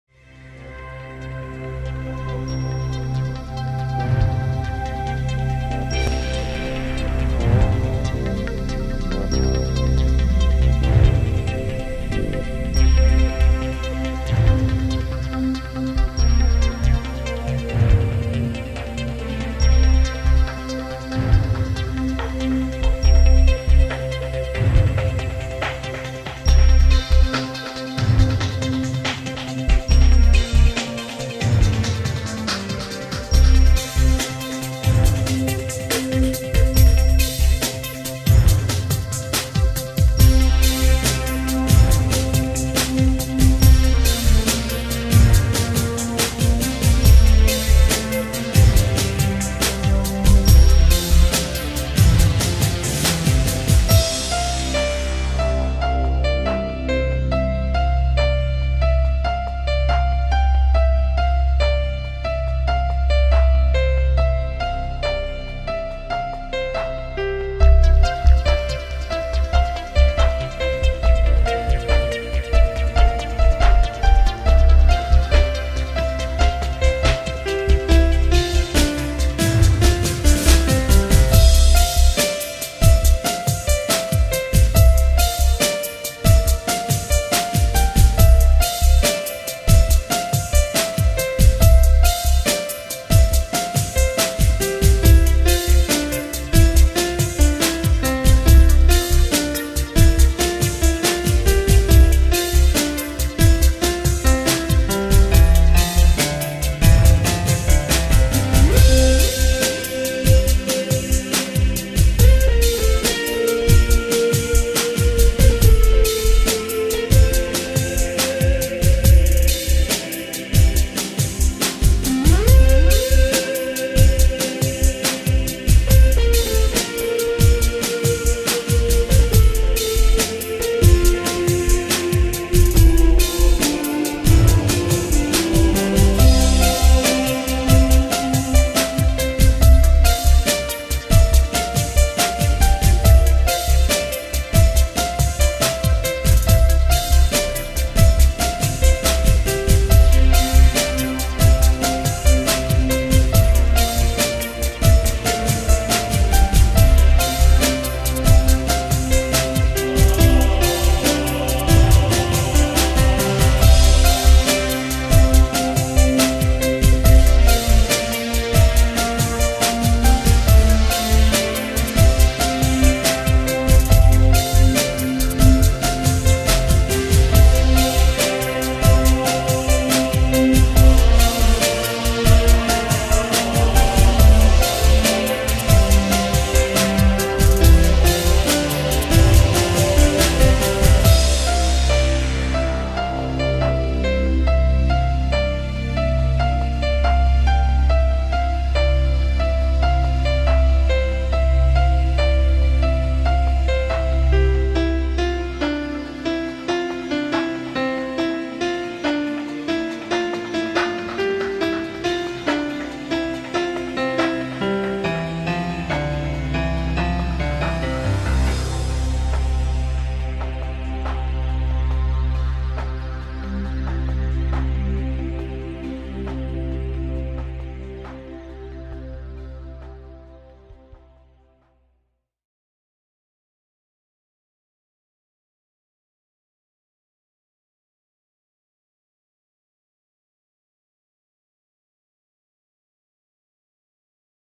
Adjustable Backlight
My homemade adjustable backlight for any camcorder or camera. It is very portable, weight only 175g with 4 AAA acumulators. Give cca 8W  of light for only 20$ (400Kč).